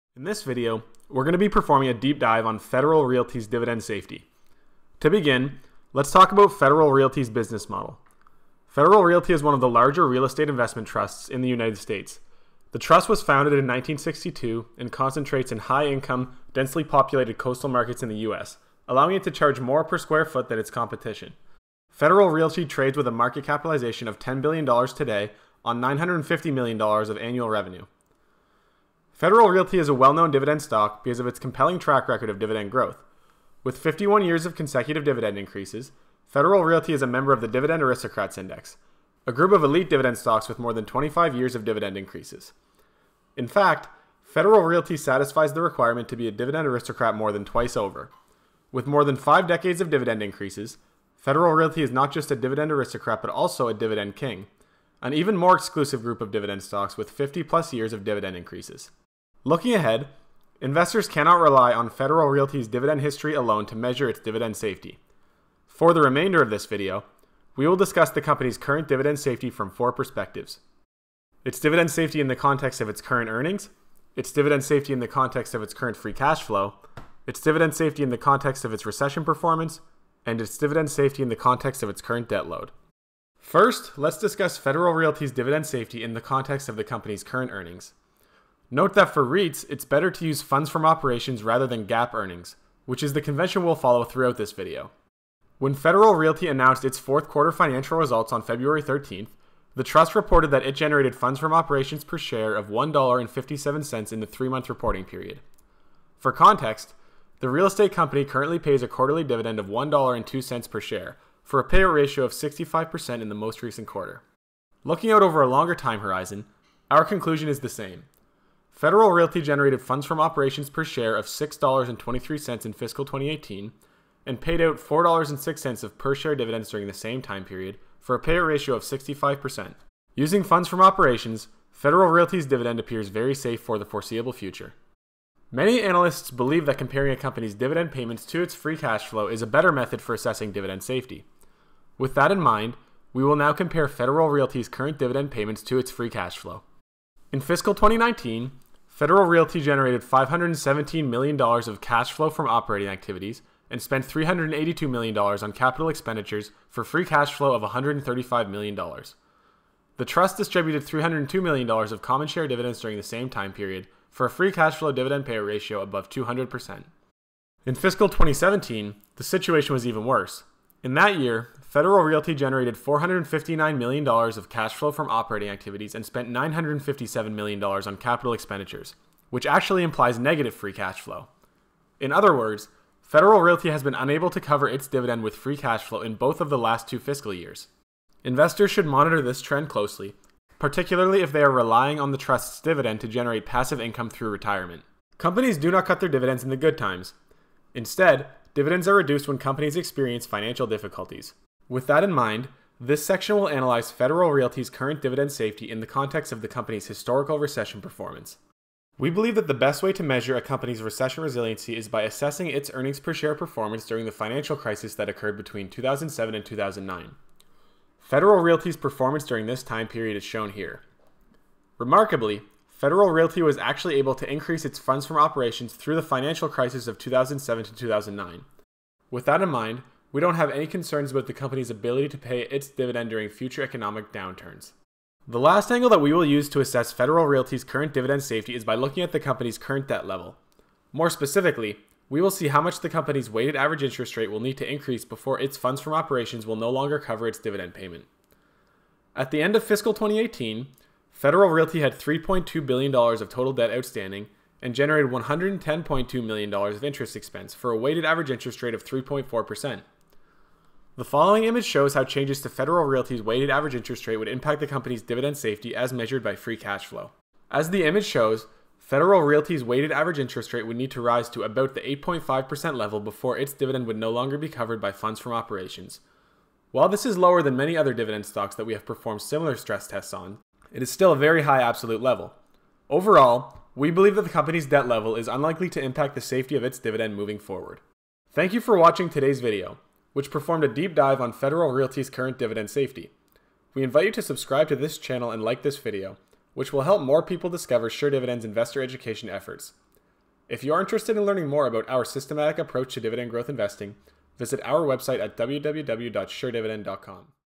How Safe is Federal Realty's Dividend?
In this video, we perform a deep dive on Federal Realty’s dividend safety.

To begin, let’s talk about Federal Realty’s business model. Federal Realty is one of the larger real estate investment trusts (REITs) in the United States. The trust was founded in 1962 and concentrates in high-income, densely-populated coastal markets in the US, allowing it to charge more per square foot than its competition. Federal Realty trades with a market capitalization of $10 billion today on $950 million in annual revenue.

Federal Realty is a well-known dividend stock because of its compelling track record of dividend growth. With 51 years of consecutive dividend increases, Federal Realty is a member of the Dividend Aristocrats Index, a group of elite dividend stocks with more than 25 years of consecutive dividend increases. You can download our free list of Dividend Aristocrats

In fact, Federal Realty satisfies the requirement to be a Dividend Aristocrat more than twice over. With more than five decades of dividend increases, Federal Realty is not just a Dividend Aristocrat but also a Dividend King, an even more exclusive group of dividend stocks with 50+ years of dividend increases. You can download our free list of Dividend Kings

Federal Realty’s Dividend Safety Relative to Earnings

First, let’s discuss Federal Realty’s dividend safety in the context of the company’s current earnings. Note that for REITs, it is better to use funds from operations rather than GAAP earnings, which is a convention we’ll follow throughout this video.

When Federal Realty announced its fourth quarter financial results on February 13th, the trust reported that it generated funds from operations per share of $1.57 in the three-month reporting period. For context, the real estate company currently pays a quarterly dividend of $1.02 for a payout ratio of 65% in the most recent quarter.

Looking out over a longer time horizon, our conclusion is the same. Federal Realty generated funds from operations per share of $6.23 in fiscal 2018 and paid out $4.06 of per-share dividends during the same time period for a payout ratio of 65%.

Federal Realty’s Dividend Safety Relative to Free Cash Flow

In fiscal 2019, Federal Realty generated $517 million of cash flow from operating activities and spent $382 million on capital expenditures for free cash flow of $135 million. The trust distributed $302 million of common share dividends during the same time period for a free cash flow dividend payout ratio above 200%.

In fiscal 2017, the situation was even worse. In that year, Federal Realty generated $459 million of cash flow from operating activities and spent $957 million on capital expenditures, which actually implies negative free cash flow. In other words, Federal Realty has been unable to cover its dividend with free cash flow in both of the last two fiscal years. Investors should monitor this trend closely, particularly if they are relying on the trust’s dividend to generate passive income through retirement.

Federal Realty’s Dividend Safety Relative to Recession Performance

We believe that the best way to measure a company’s recession resiliency is by measuring its earnings-per-share performance during the financial crisis that occurred between 2007 and 2009. Federal Realty’s performance during this time period is shown here:

• 2007 funds from operations: $3.66
• 2008 funds from operations: $3.87
• 2009 funds from operations: $3.87
• 2010 funds from operations: $3.88
• 2011 funds from operations: $4.00
• 2012 funds from operations: $4.31

Remarkably, Federal Realty was actually able to increase its funds from operations through the financial crisis of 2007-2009. With that in mind, we do not have any concerns about the company’s ability to continue paying its dividend during future economic downturns.

Federal Realty’s Dividend Safety Relative to Its Current Debt Load

At the end of fiscal 2018, Federal Realty had $3.2 billion of total debt outstanding and generated $110.2 million of interest expense for a weighted average interest rate of 3.4%.

The image contained in the video shows that Federal Realty’s weighted average interest rate would need to rise to about the 8.5% level before its dividend would no longer be covered by funds from operations. While this is lower than many of the other dividend stocks that we have performed similar stress tests on, it is still a very high absolute level. Overall, we believe that the company’s debt level is unlikely to impact the safety of its dividend moving forward.